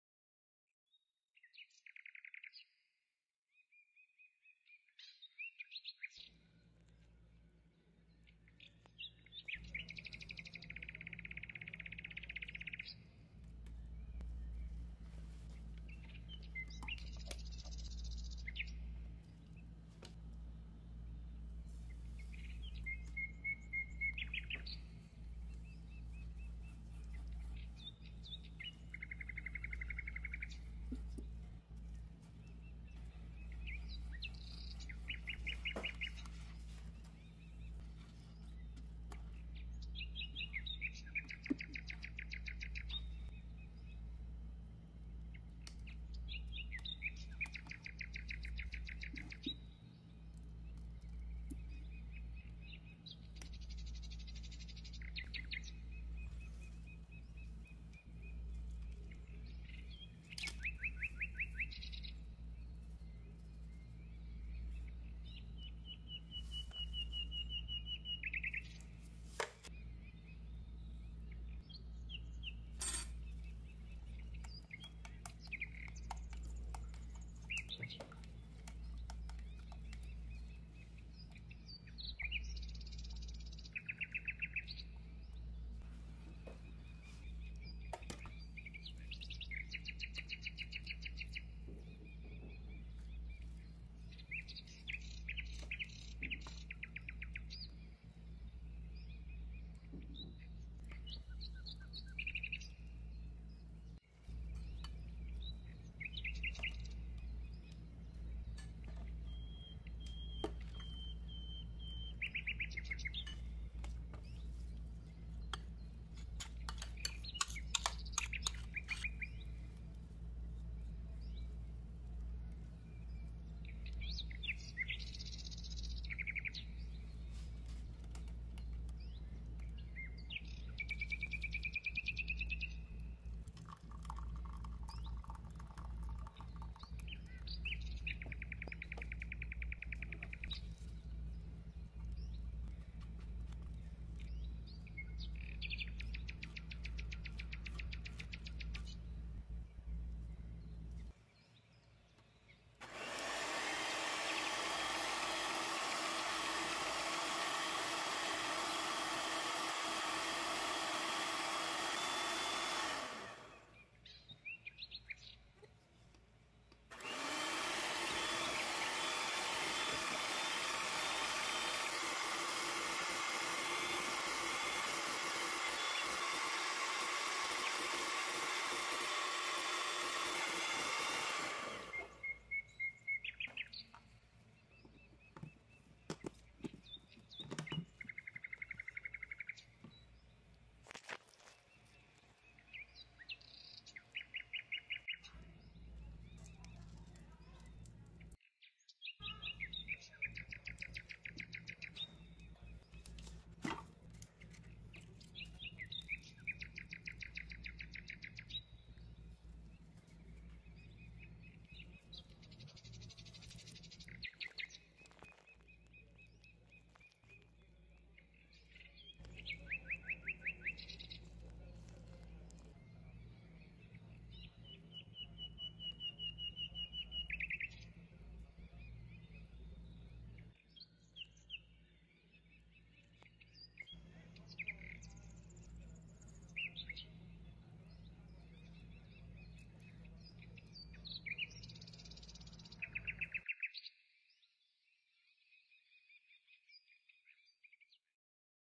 بمكونين فقط لاغير سوفت ايس كريم/ايس كريم ماكدونالدز الناعم
المقادير
٧٠٠مللي كريمه خفق
٤٠٠جرام سكر